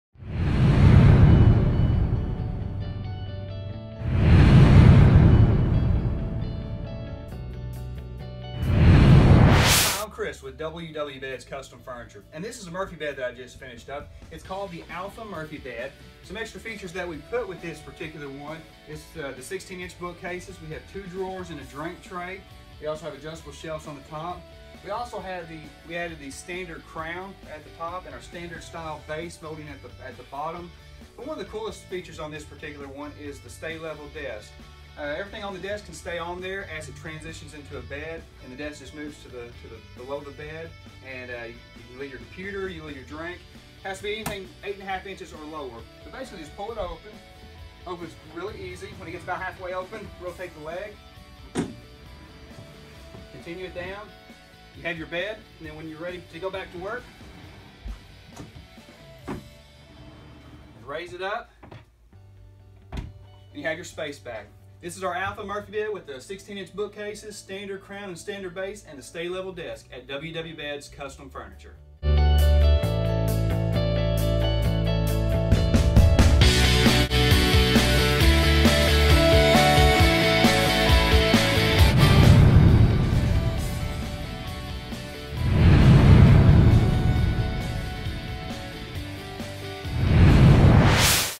Alpha Murphy Bed With Stay Level Desk
This murphy bed has a desk that stays level as it moves into the bed position.  No need to clear off the desk.  We can add this to most wall bed styles.